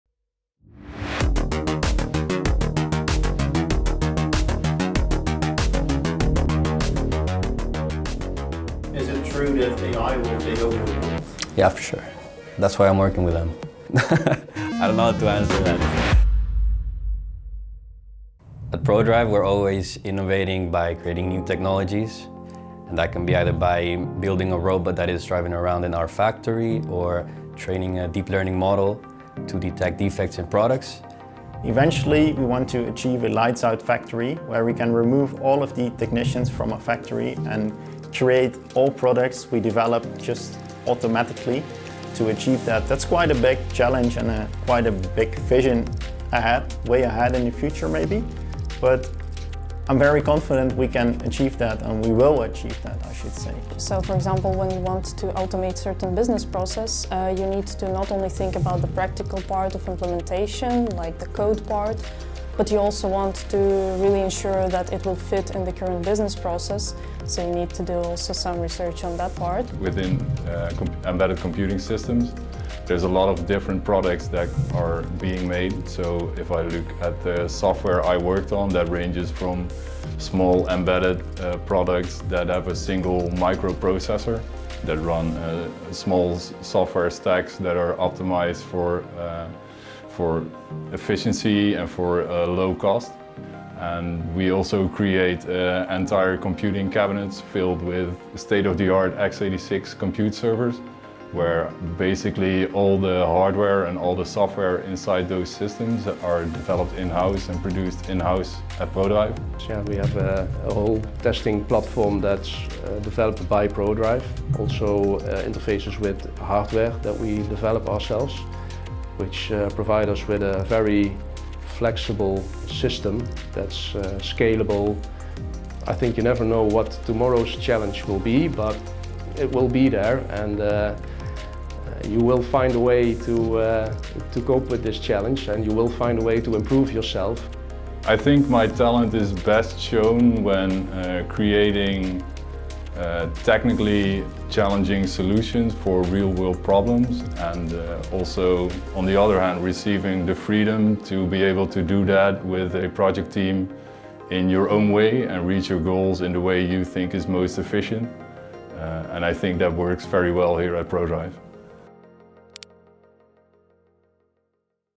Prodrive Technologies - Software Engineering
Want to be part of something that genuinely helps keep the world running? Prodrive Technologies is currently looking for new Software Engineers to become part of our global team of trailblazers!

Put your passion for technology to work, and be rewarded with flexibility, recognition, training, and personal development opportunities. Plus, a range of employee benefits designed with your wellbeing in mind.